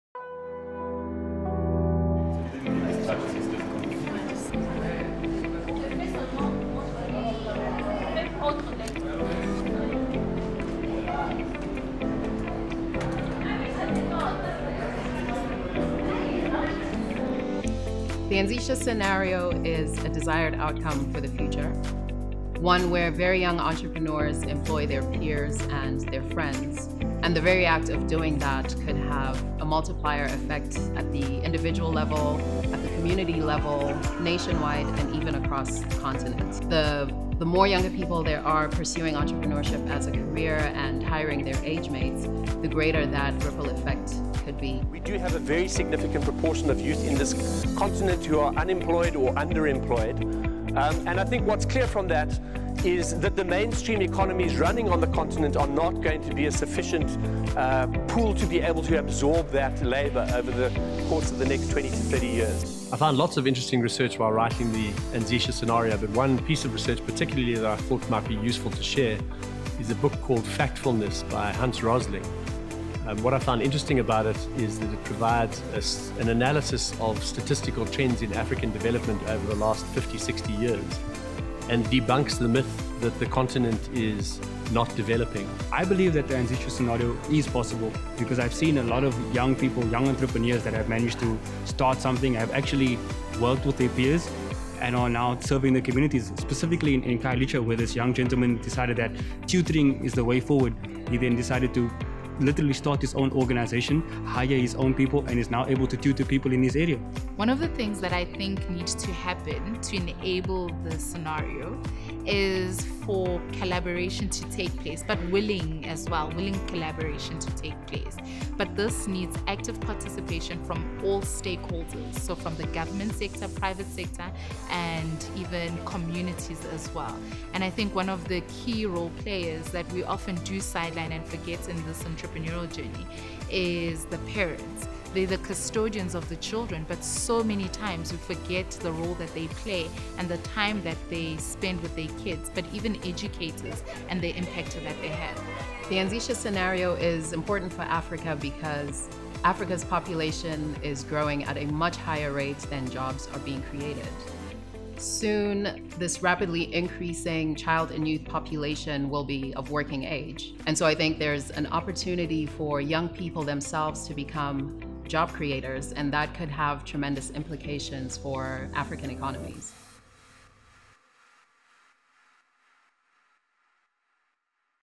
The Anzisha Prize Scenario | Anzisha Prize Fellowship
The Anzisha Scenario is a campaign by the Anzisha Prize in which we imagine a future where, in response to the challenge of youth unemployment, young Africans of high potential choose entrepreneurship as a career.

The campaign seeks to engage multiple stakeholders - parents, teachers, investors, and others - to collectively envision whether and how enabling very young entrepreneurs could impact the employment situation in Africa.

The Anzisha Scenario is not a predetermined outcome: it is a desired one. Before determining practical recommendations of how to achieve this vision, it is necessary to understand the current reality, and then consider the feasibility of the Anzisha Scenario by identifying drivers, barriers, and opportunities.